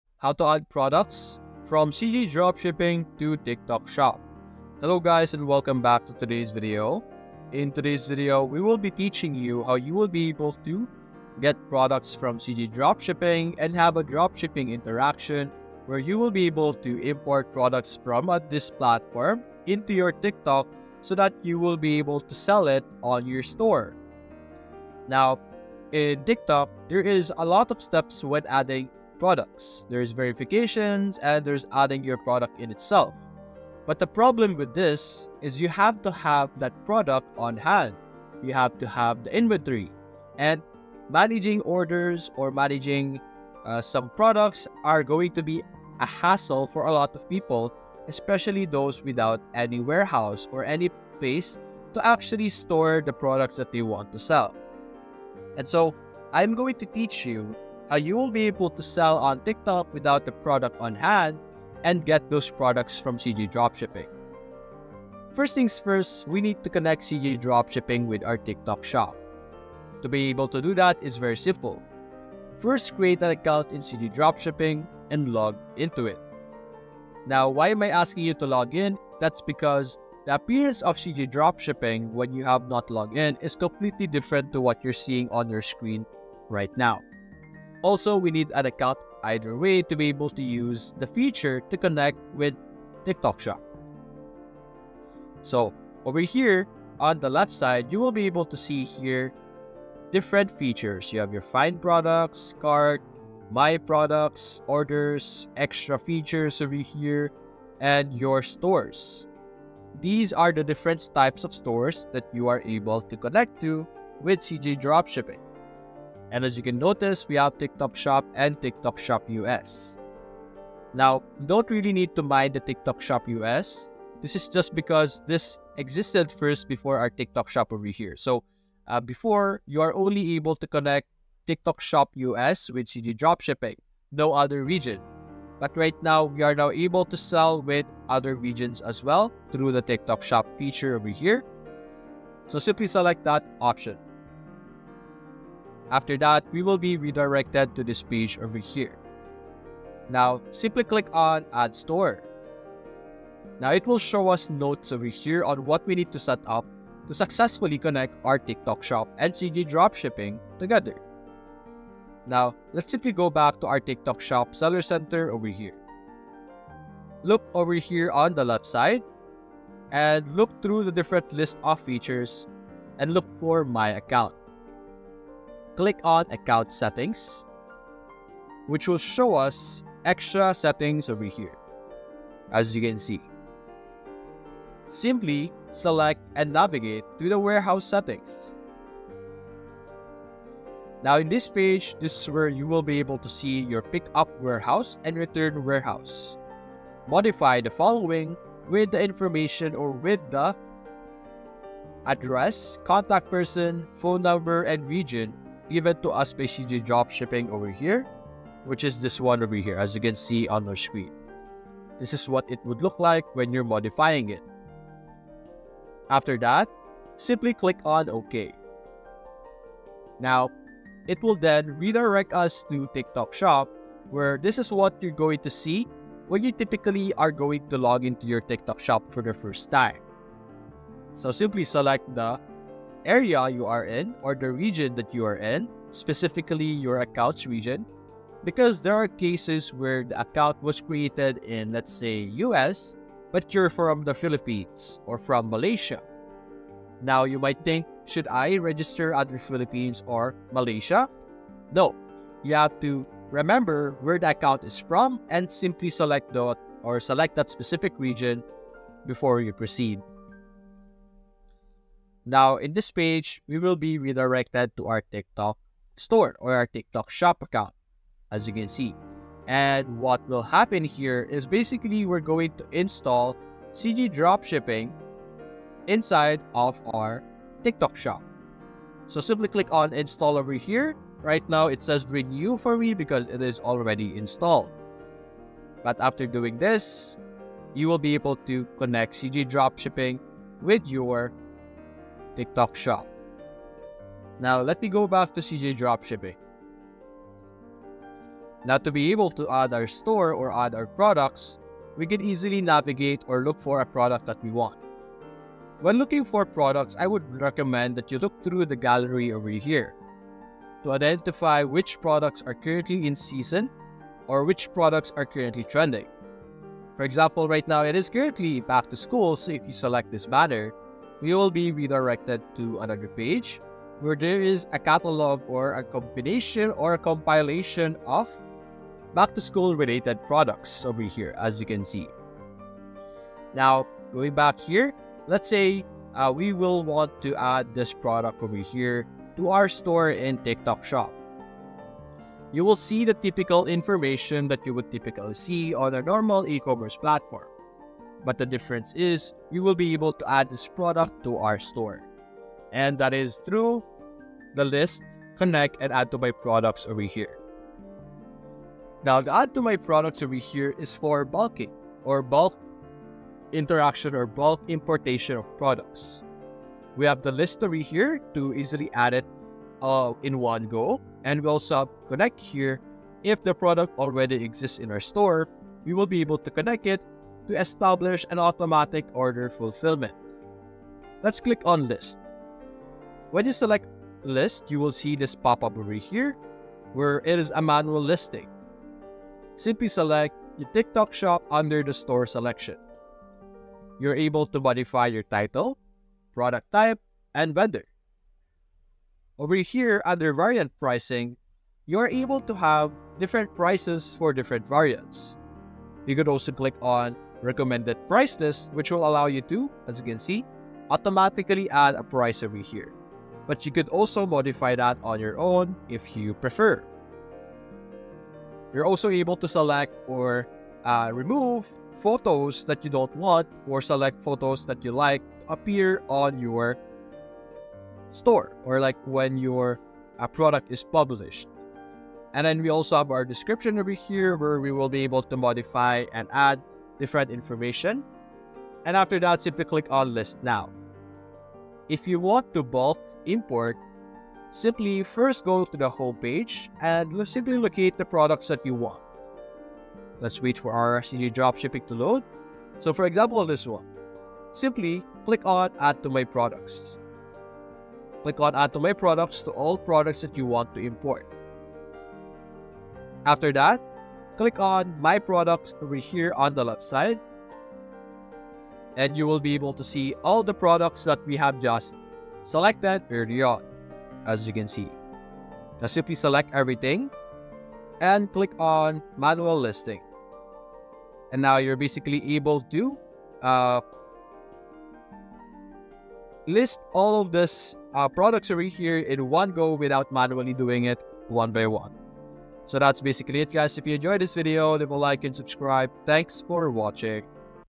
How to Add Products From CJ Dropshipping to Tiktok (Full Guide)
Learn how to add products from CJ Dropshipping to Tiktok with this full guide. Boost your e-commerce sales with this step-by-step tutorial!